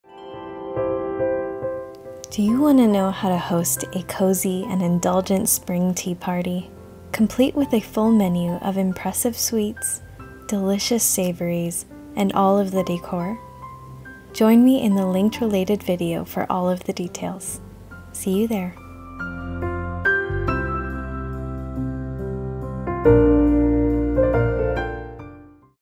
Cozy Spring Afternoon Tea Party
A snippet from a longer video about how to host a cozy Spring afternoon tea party. In the longer video, I include recipes, decor, and menu, and planning details. I look forward to seeing you there!